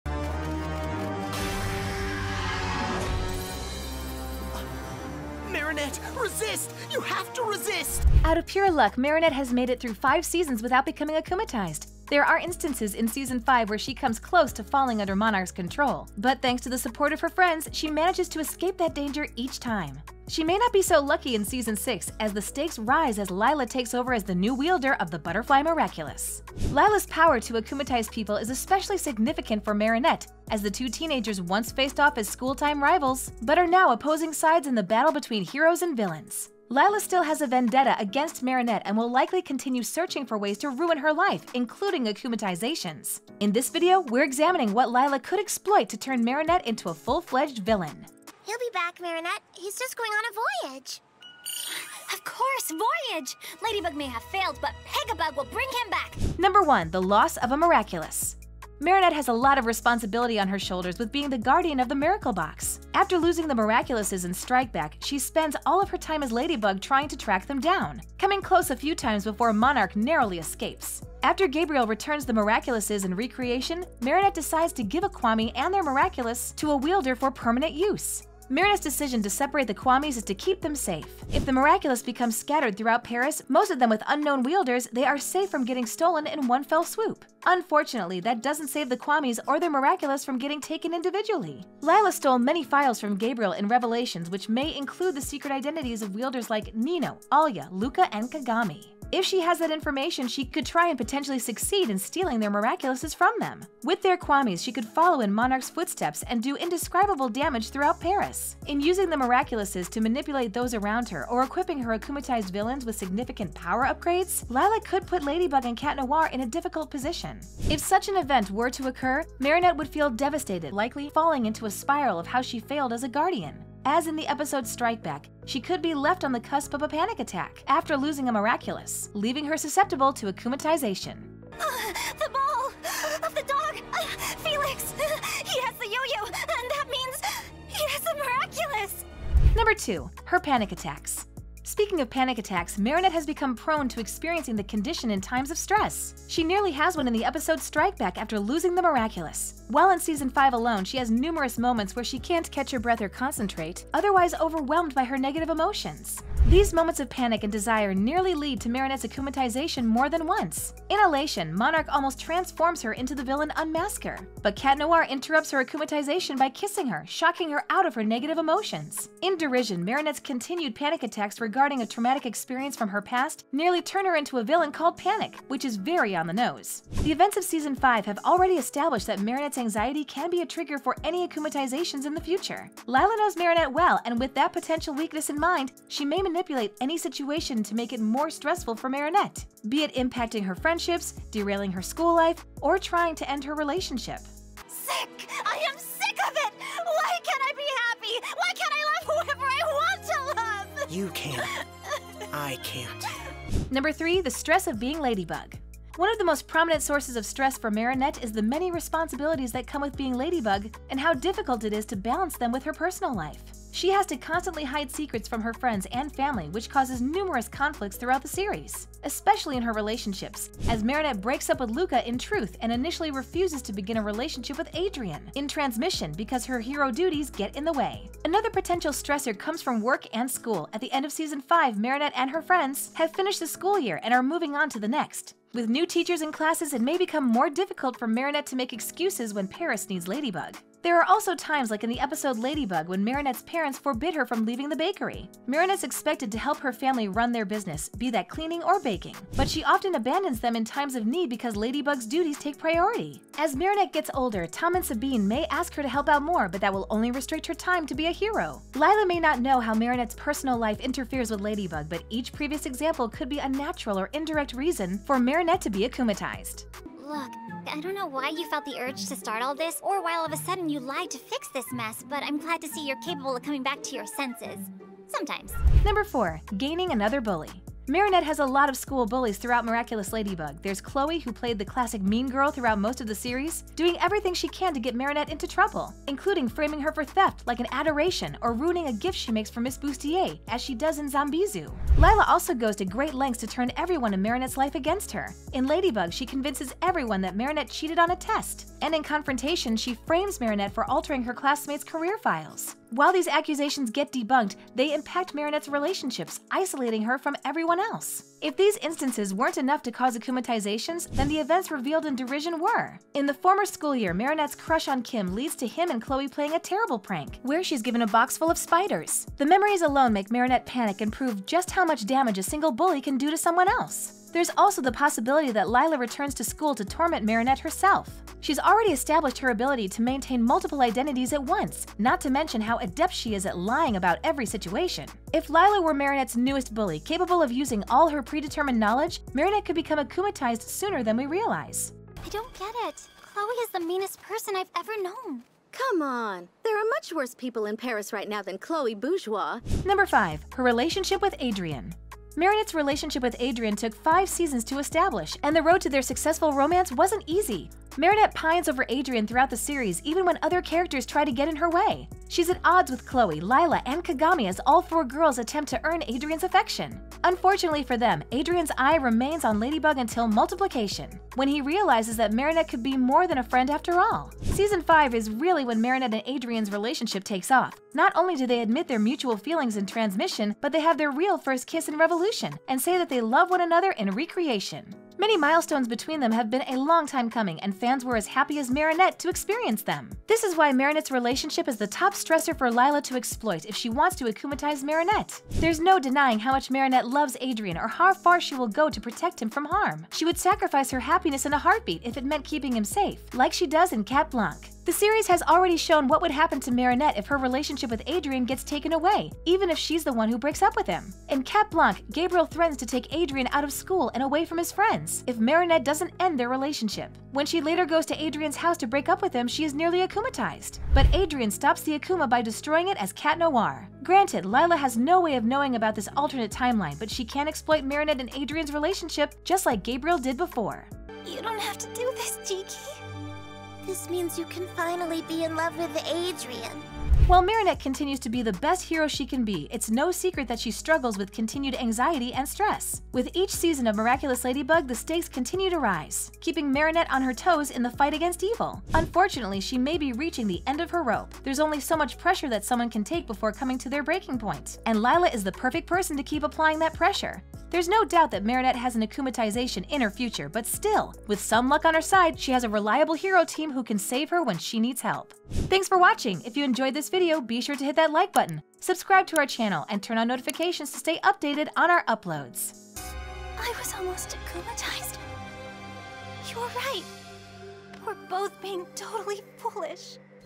Marinette Finally Turns Into A Real Akumatized Villain In Season 6?!
Marinette Finally Turns Into An Akumatized Villain In Miraculous Ladybug Season 6?!

Out of pure luck, Marinette has made it through five seasons without becoming akumatized. There are instances in season five when she comes close to falling under Monarch’s control, but thanks to the support of her friends, she manages to escape that danger each time! She may not be so lucky in season six, as the stakes rise as Lila takes over as the new wielder of the Butterfly Miraculous.

Lila’s power to akumatize people is especially significant for Marinette, as the two teenagers once faced off as school-time rivals but are now opposing sides in the battle between heroes and villains. Lila still has a vendetta against Marinette and will likely continue searching for ways to ruin her life, including akumatizations! In this video, we’re examining what Lila could exploit to turn Marinette into a full-fledged villain.